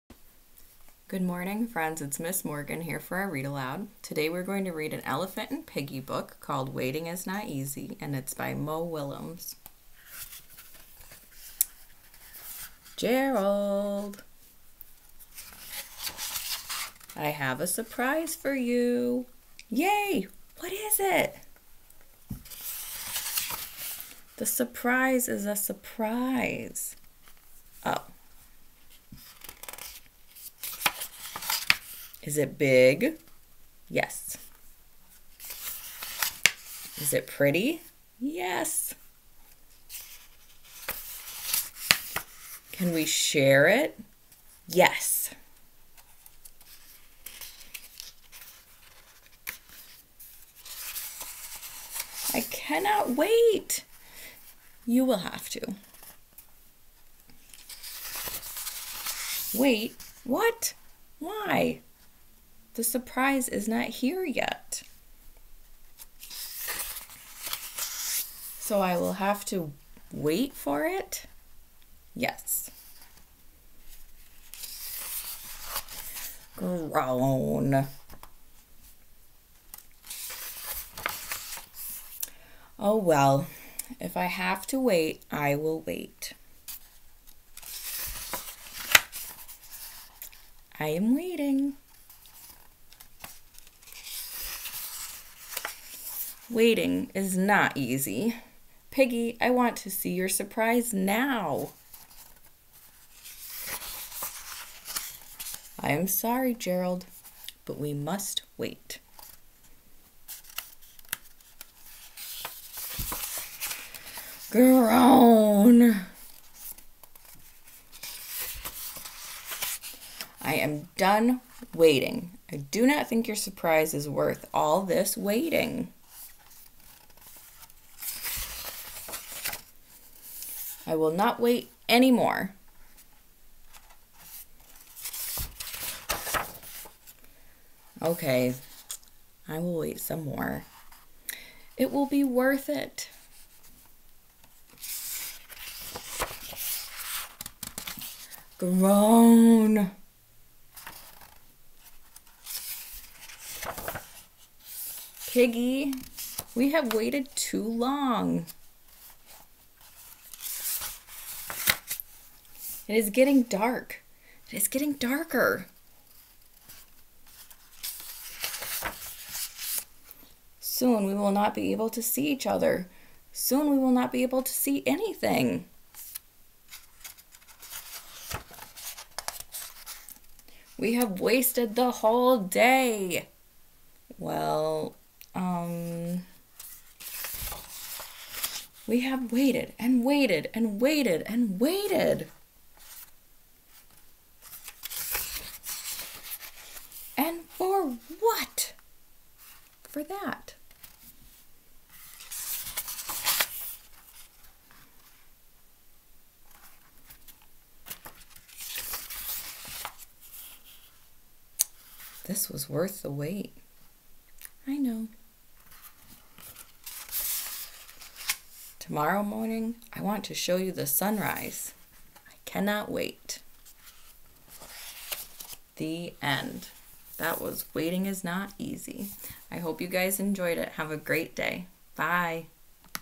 Mrs. Hicks' read aloud "Waiting Is Not Easy!" by Mo Willems
Today we area reading an Elephant and Piggie Book called "Waiting Is Not Easy!" by Mo Willems